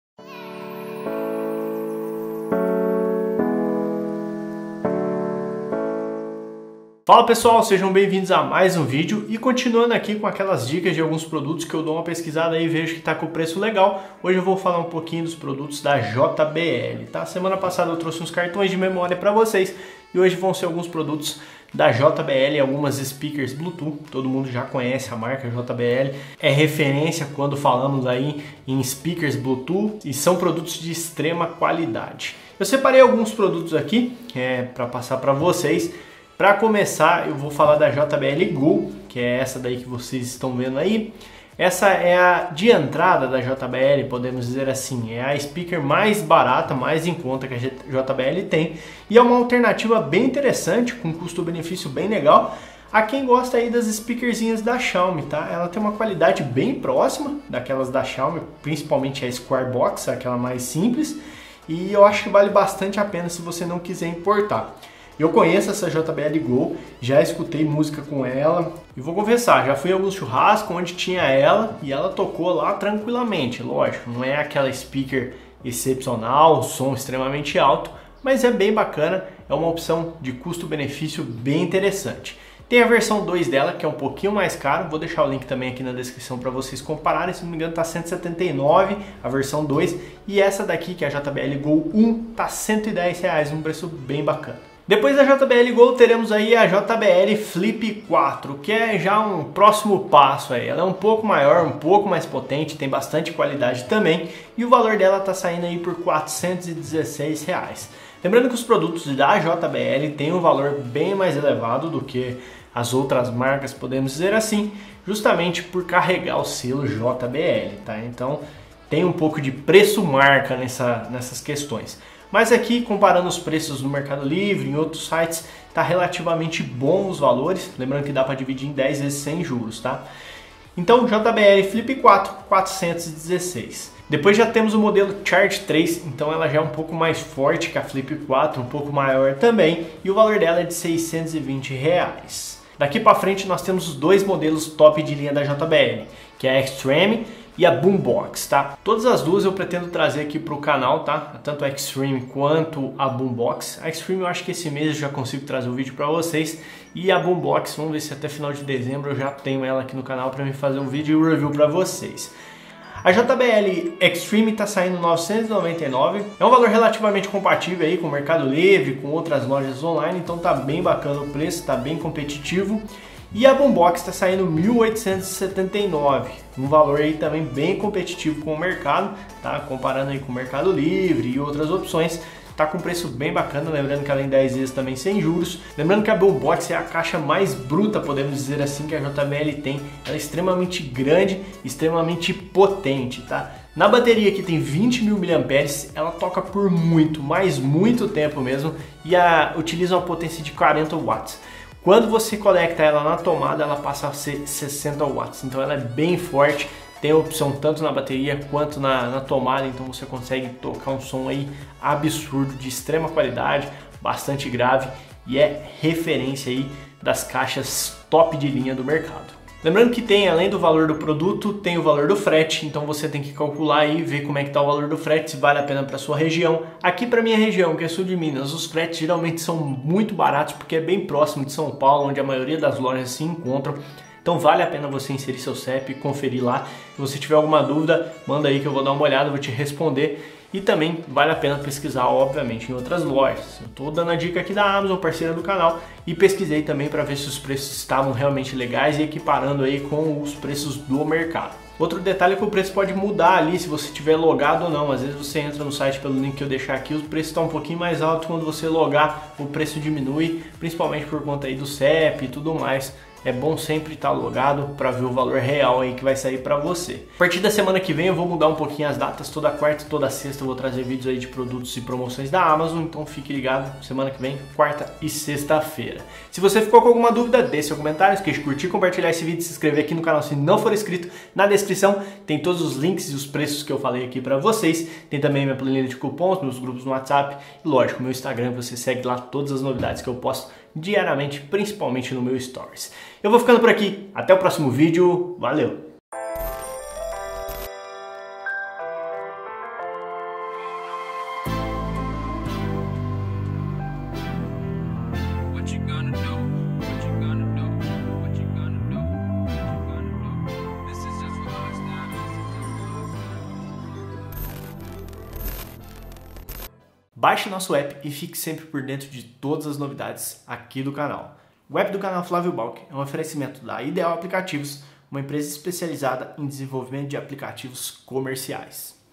SPEAKERS JBL COM PREÇO BACANA (PARA TODOS OS GOSTOS E BOLSOS RSRS)
Vídeo produzido em parceria com Amazon Brasil.

Links dos produtos (pode ser necessário logar para chegar nos valores citados):

JBL GO R$ 110,62

JBL GO 2 R$ 169,50 (baixou mais)

JBL FLIP 4 R$ 416,74

JBL CHARGE 3 R$ 620,00

JBL XTREME R$ 999,00

JBL BOOMBOX R$ 1.879,00

(Estamos adicionando produtos diariamente)

Vai IMPORTAR? Acesse pelos links abaixo:

O Canal tem parcerias com as seguintes lojas:

Amazon Brasil

Curta, compartilhe e comentem!